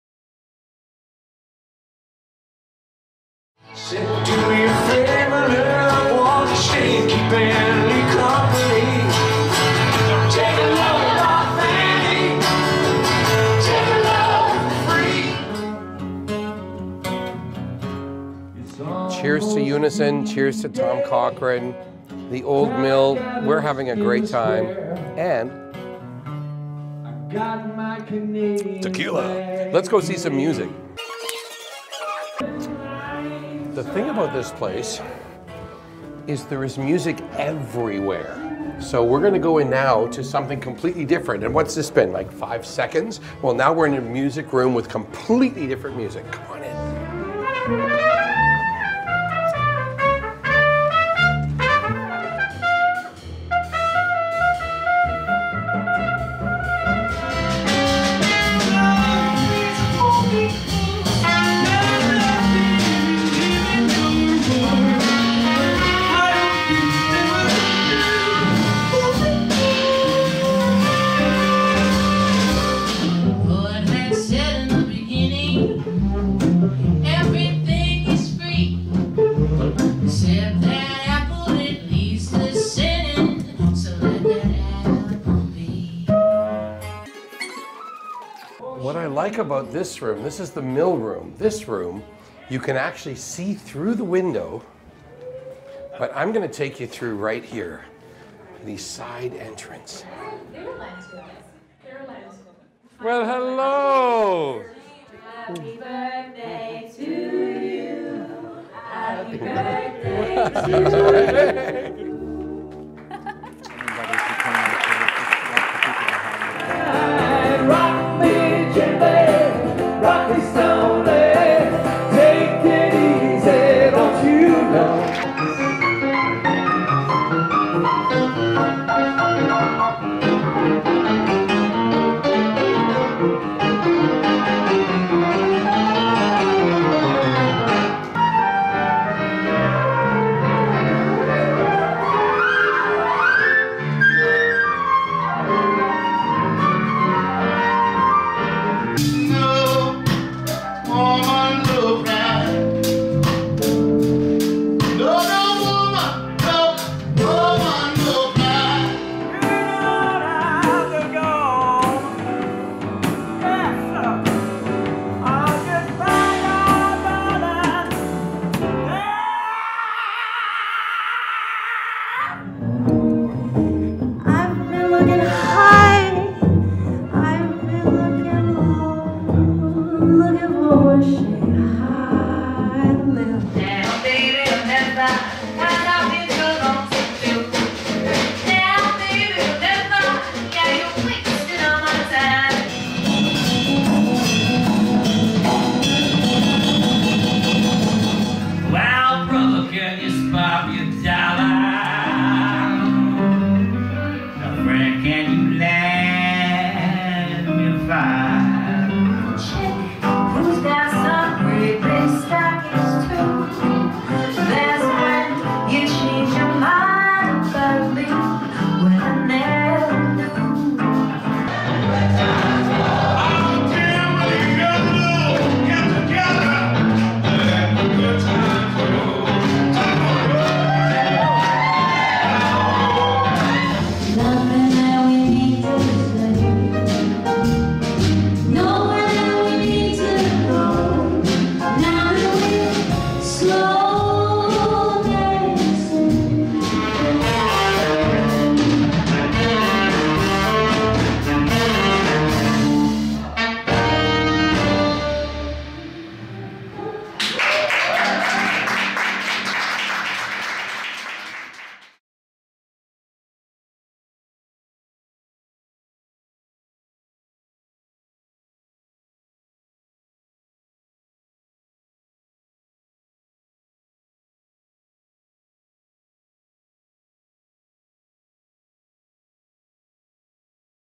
Caravan of Music - UNISON FUND 2022 Highlights
The 2023 Caravan of Music is April 12 7:30pm at Old Mill Toronto
in advance

These are highlights from 2019, pre-lockdown. This year we have 24 bands in 12 rooms.

Thanks to Keith Holding for supplying the footage!
Read about this wonderful music biz charity